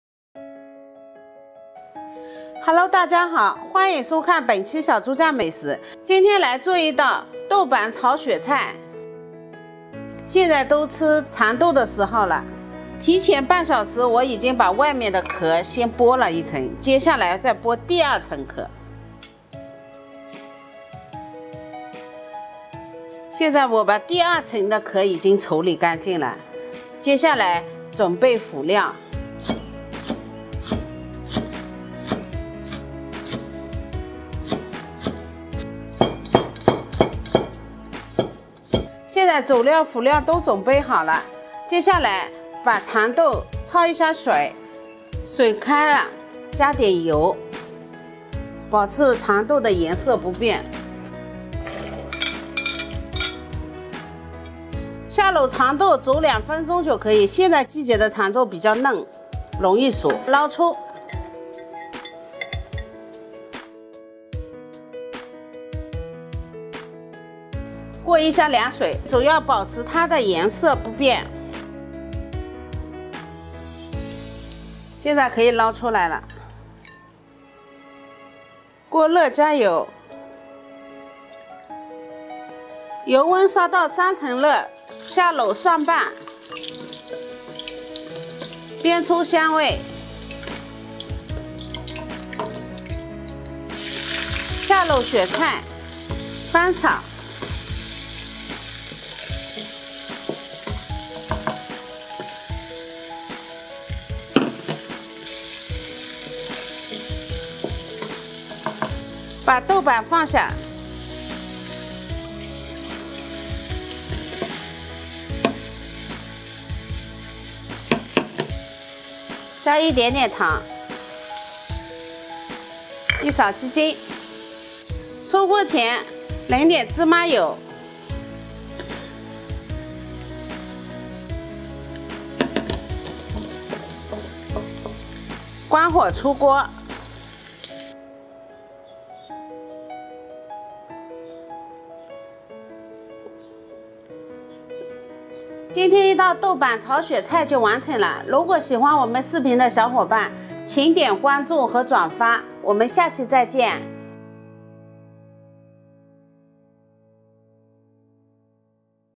雪菜炒豆瓣家常做法做出酒店味道，一上桌好吃就停不下来
美食 美食博主 中国美食 家常菜 地方家常菜欢迎来到小珠酱美食，烹饪就像爱一样，要么全心全意投入，要么完全放弃。